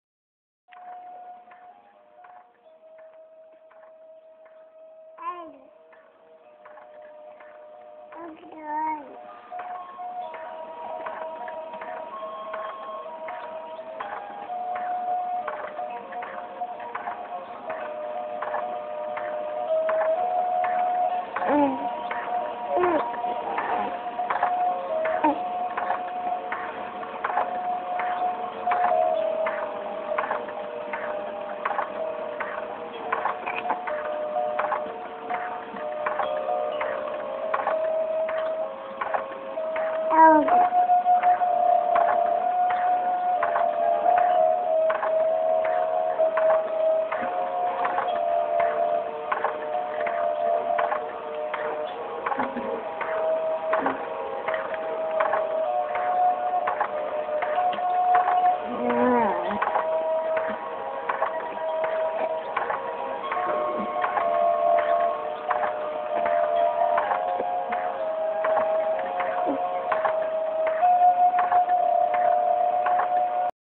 Isabella loves her swing 2
Isabella's in her swing, enjoying the ride.  She's even talking to it a few times